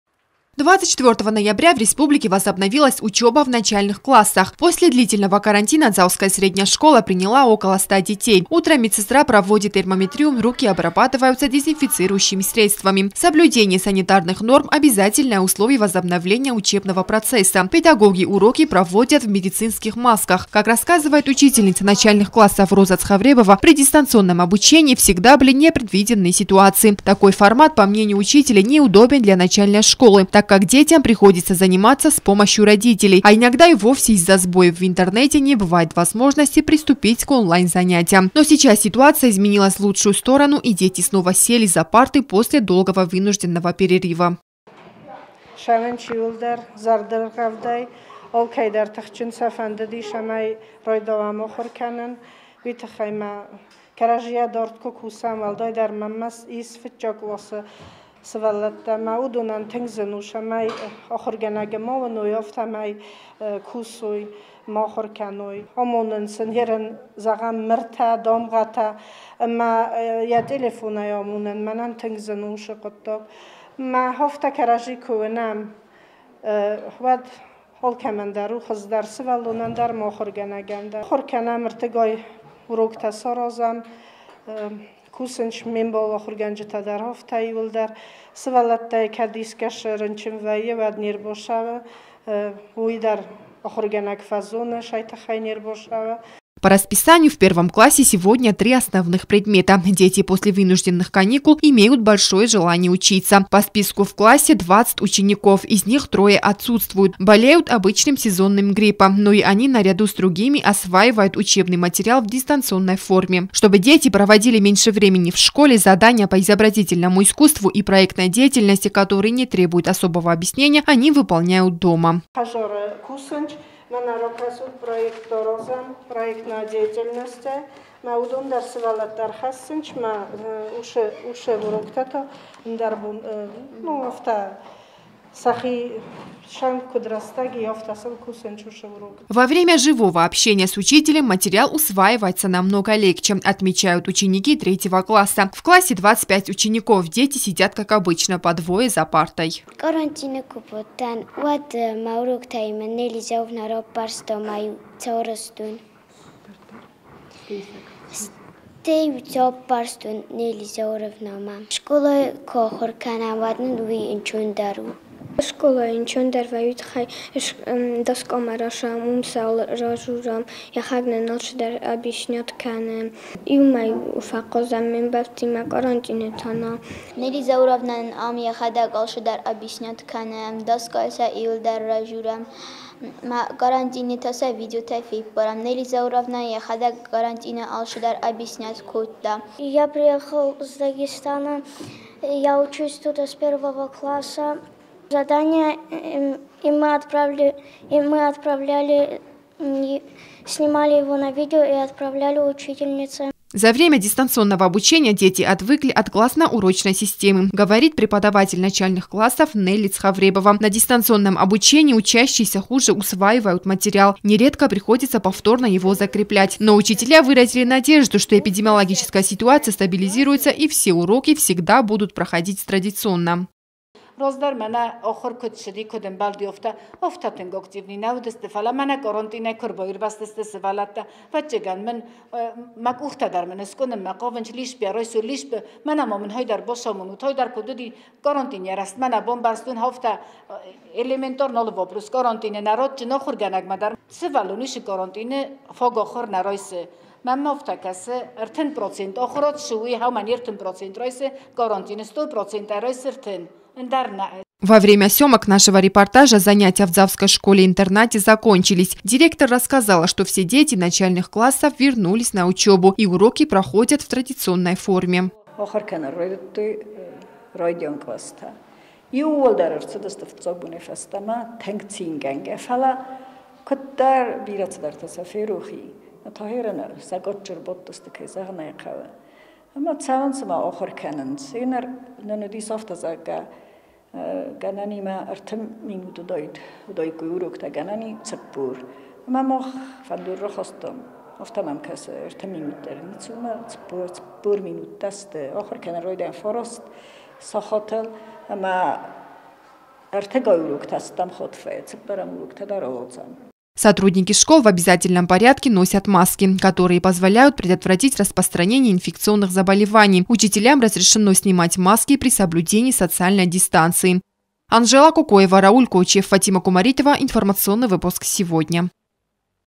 24 ноября в Цхинвале возобновилась учеба в начальных классах
Такое решение принято в связи с неудобствами при дистанционном обучении.  Педагоги отмечают, что такой формат представляет определенную сложность для учеников с 1-го по 4-ый классы.  Ход учебного процесса в Дзауской школе проверила наша съемочная группа.